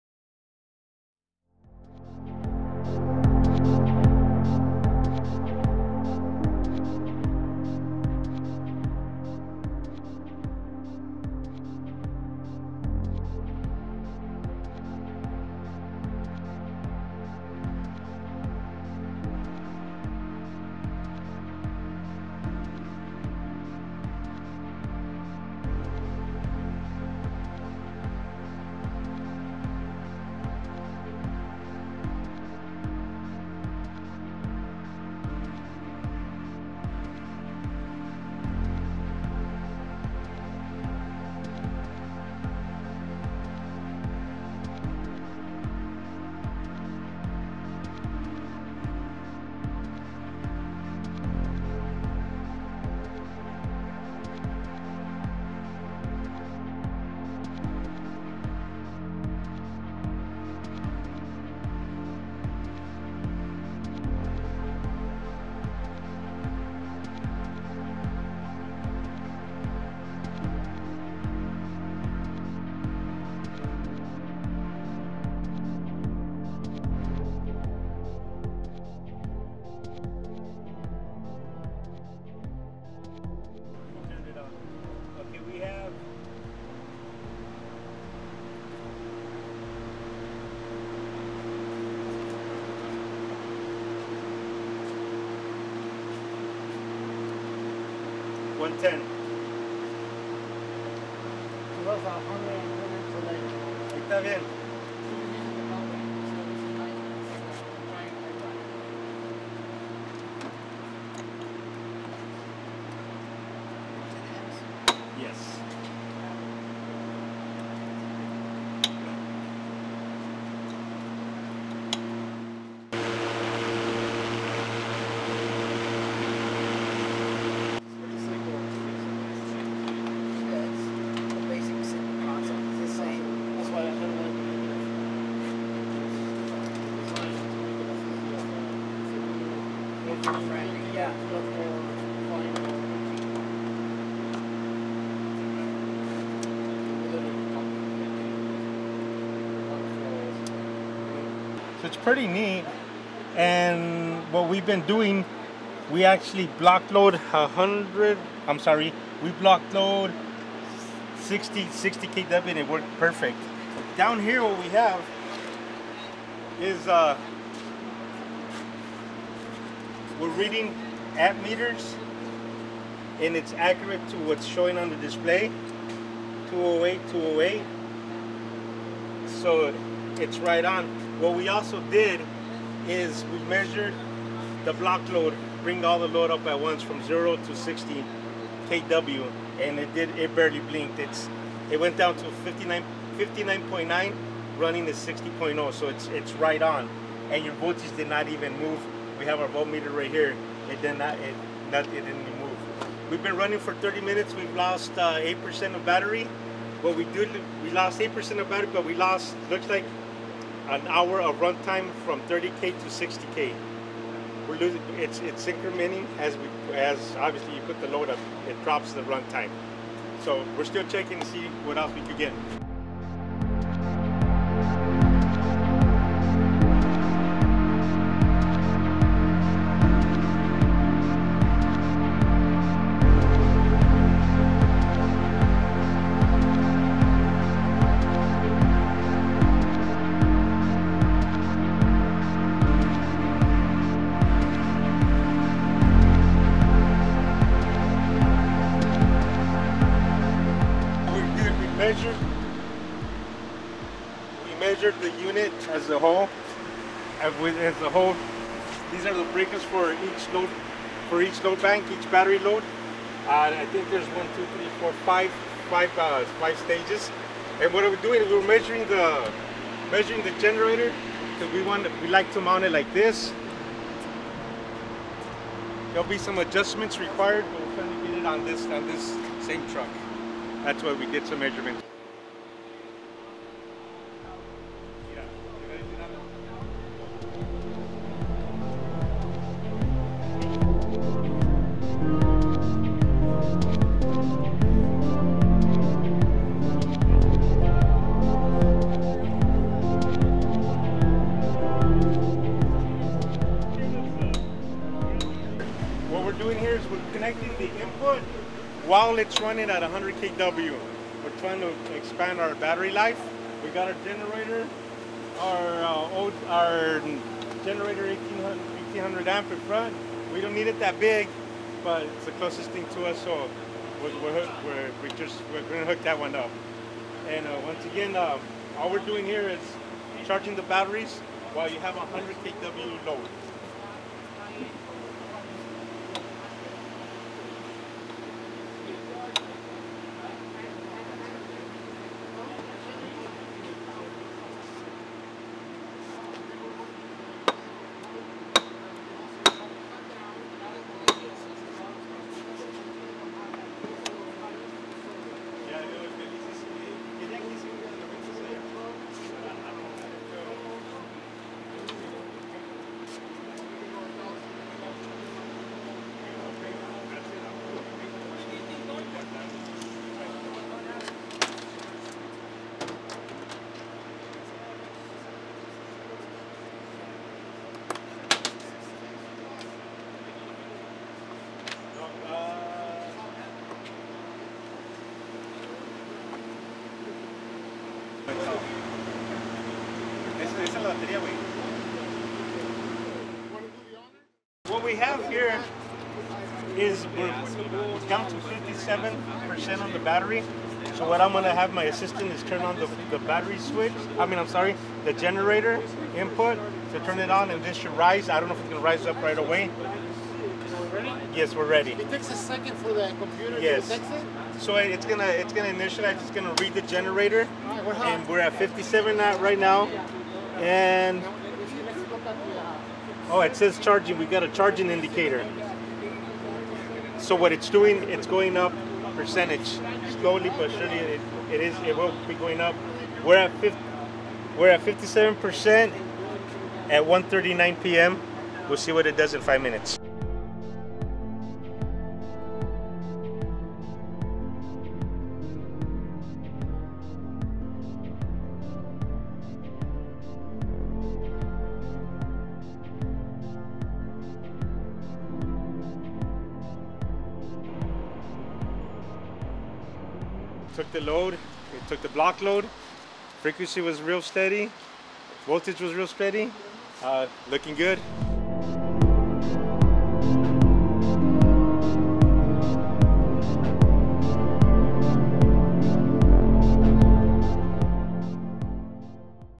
Saniset Fleet - CleanGen™J250 Load Test demo
Excited to share our J250 electric generator Load Test at CES Power!